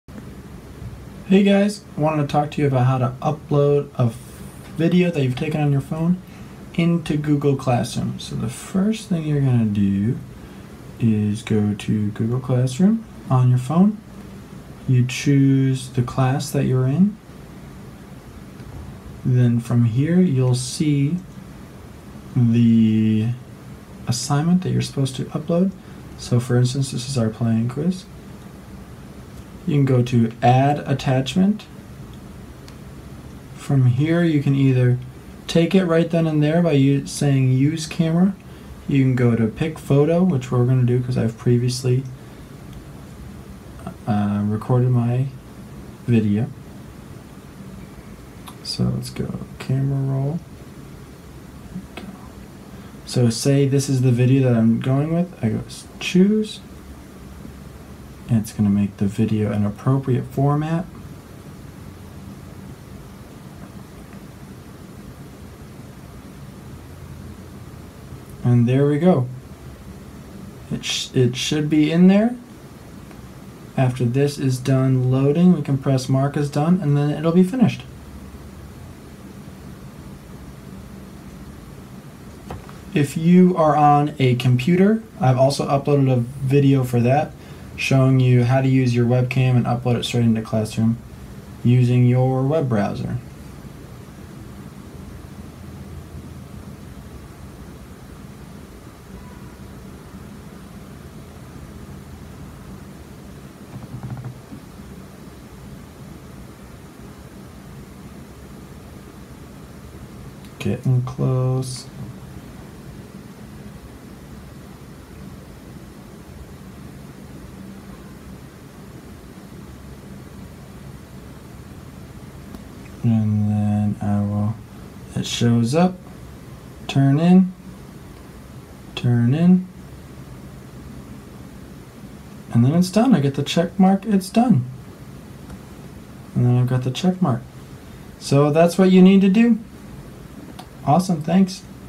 Uploading Video from Phone to Google Classroom
This video is to help band students upload their playing quiz into google classroom. Sorry for the low quality, but the content and commentary should be enough to help out!